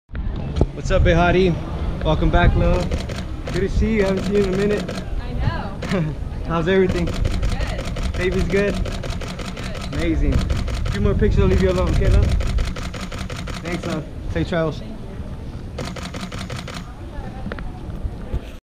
Behati Prinsloo Is Greeted By Celebrity Photographers At LAX
Friday, August 11, 2017  Supermodel Behati Prinsloo smiled for photos at LAX, dressed casual for her flight, with massive movie star sunglasses and a Nirvana t-shirt.  X17online.com